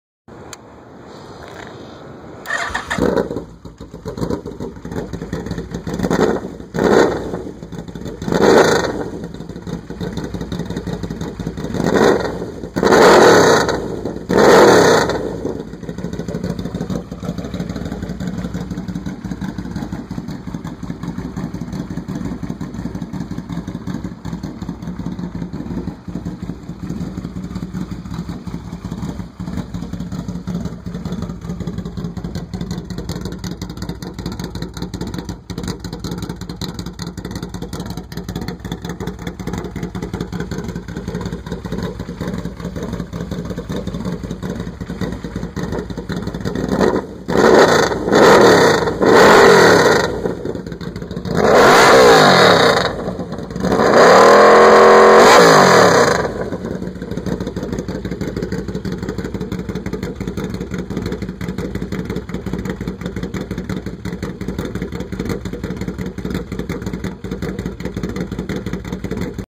2003 Yamaha Vmax 1200, UFO Pipe, RaceTech $5000
10K miles
Dynojet kit, K&N filter, UFO pipe
RaceTech cartridge emulators, Progressive fork springs
Road Ready
bad attitude!